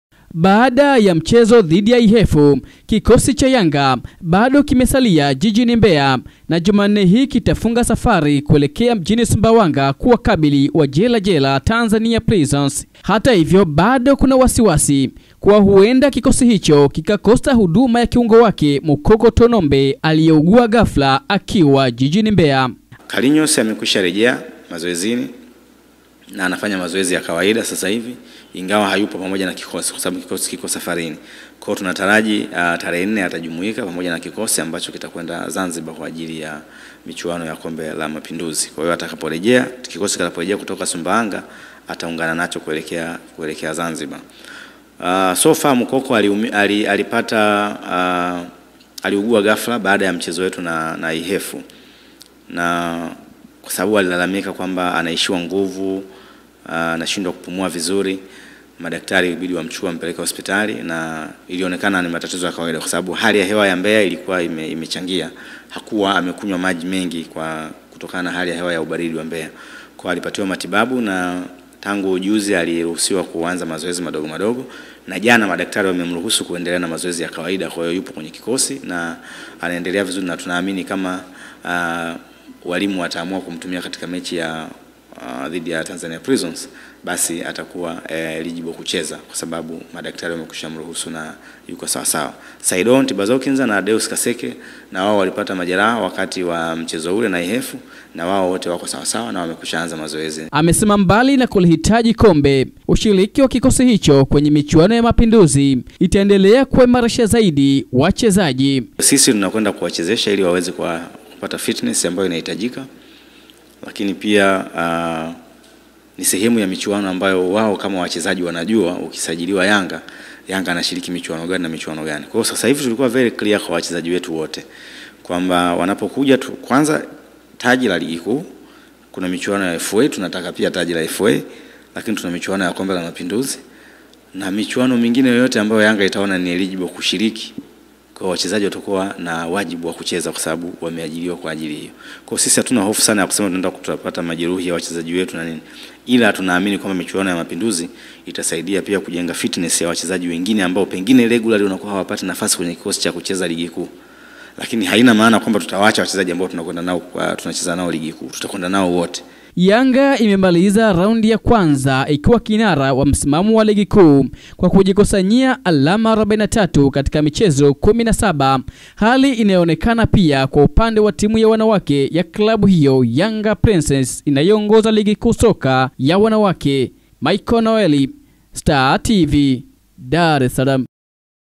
YANGA SC YAJITAPA KUPELEKA KIKOSI KAMILI MAPINDUZI CUP.
Klabu ya yanga imethibitisha kuwa baada ya mchezo wa ligi kuu dhidi ya Tanzania Prisons itakayopigwa alhamisi wiki hii katika uwanja wa Nelson Mandela Sumbawanga mkoani Rukwa, klabu hiyo itapeleka kikosi kamili kushiriki michuano ya Kombe la Mapinduzi Visiwani Zanzibar kwani malengo yao msimu huu ni kubeba mataji ya michuano yote wanayoshiriki.